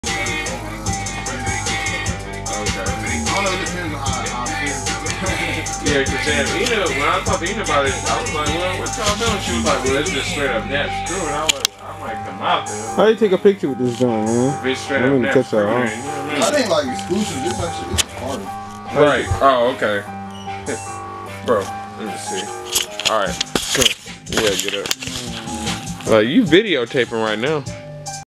RACKSTAR SESSION P.4 AT UNDERGROUND RICH STUDIOS!
ANOTHER STUDIO SESSION WITH MIKEY RACKSTAR! WHIPIN UP SOME fIRE!!!